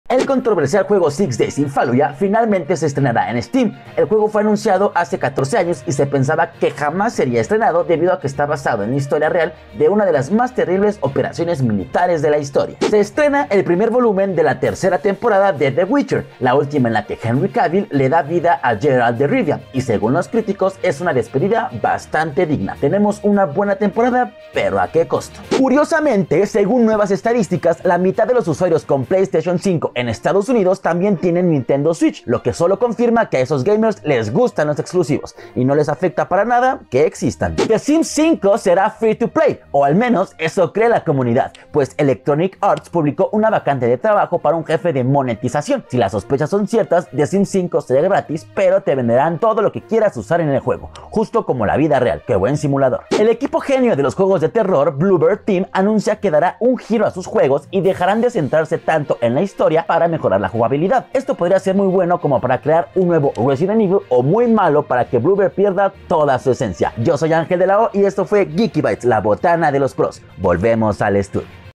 ¡GeekyBites! 🎮😎🫦
Six Days in Fallujah finalmente se estrenará en Steam | La mitad de los usuarios con PS5 en Estados Unidos también tienen Nintendo Switch | The Sims 5 será Free-to-play | Bloober Team anuncia que dará un giro a sus juegos.